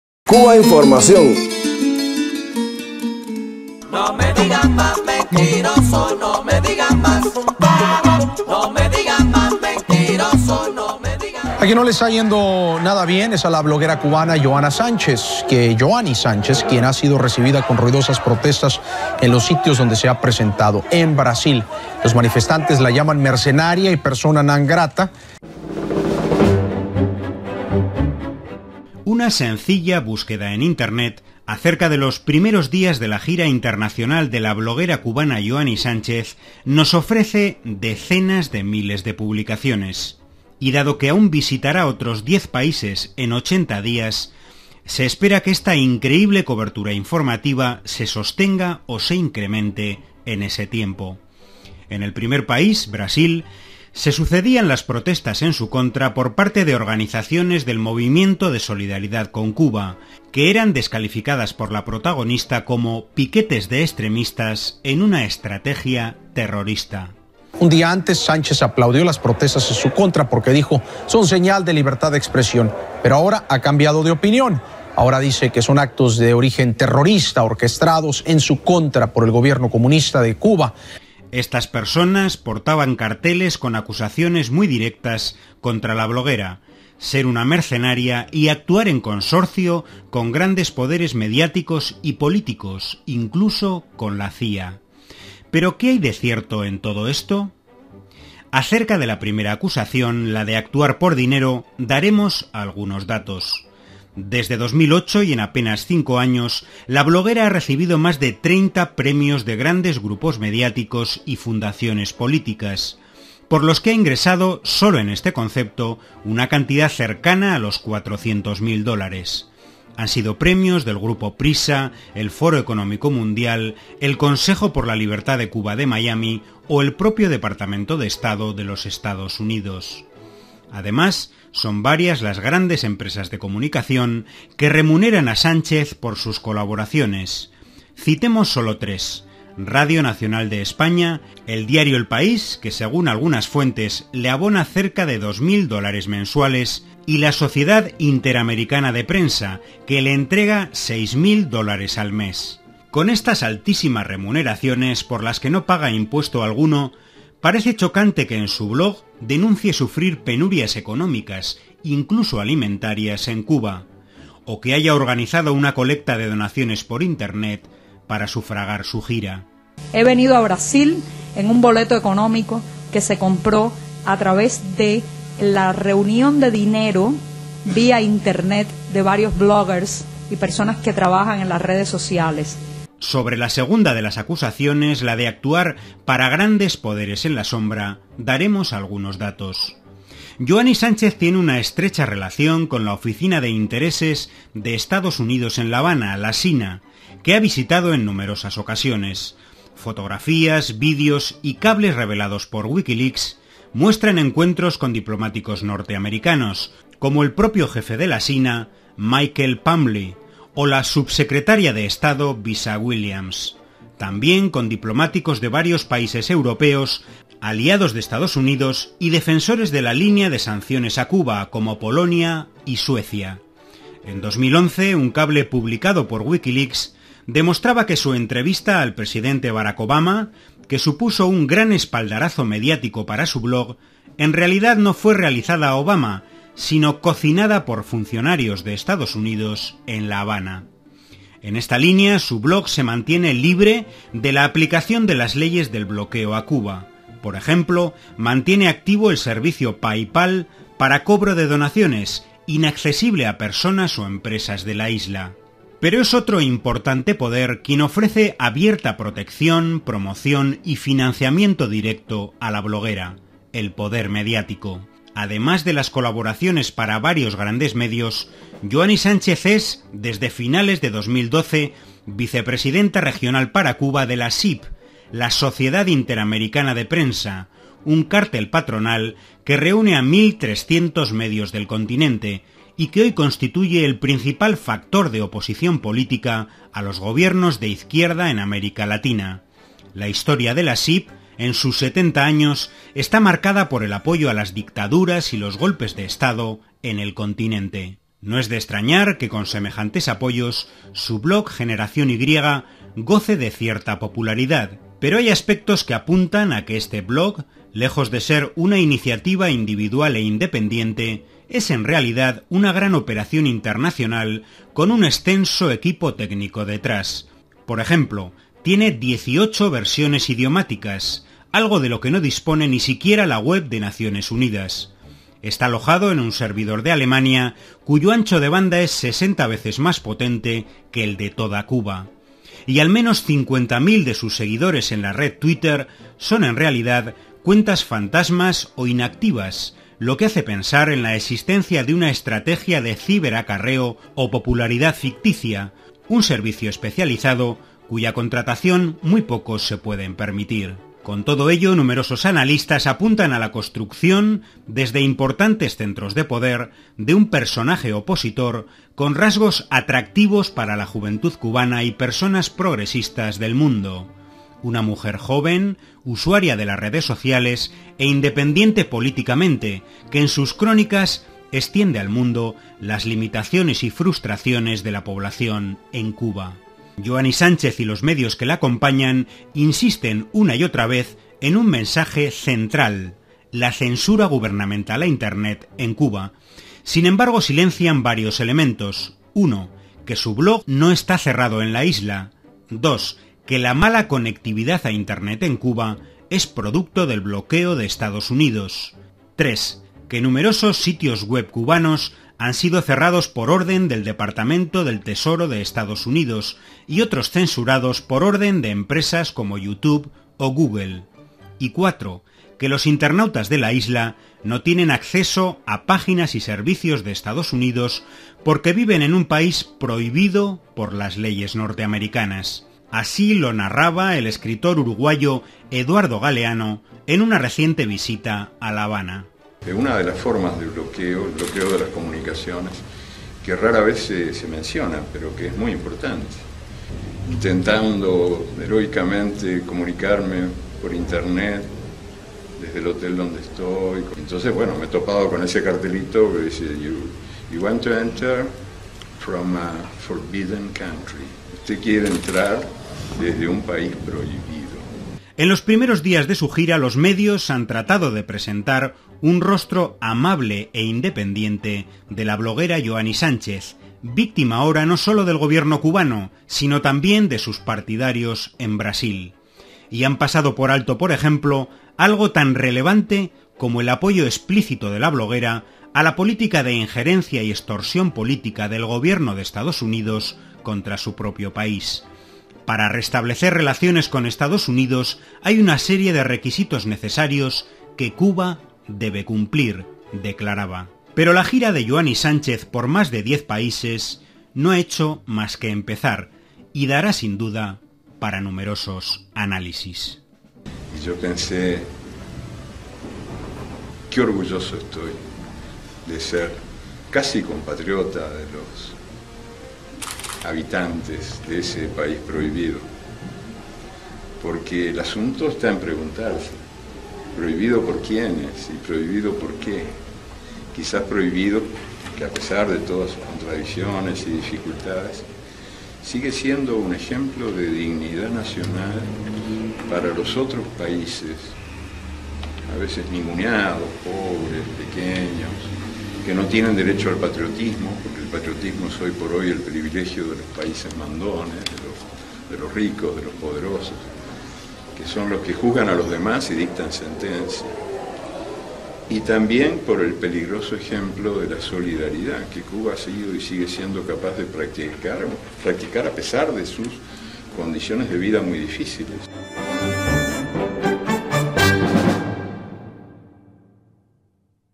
Mercenaria y agente de la CIA gritaron en Brasil a Yoani Sánchez: ¿qué hay de cierto en ello?
Varios agentes del poder mediático remuneran a Sánchez, entre ellos el diario español El País que, según algunas fuentes, le abona cerca de 2.000 dólares mensuales, y la Sociedad Interamericana de Prensa, que le entrega 6.000 dólares al mes.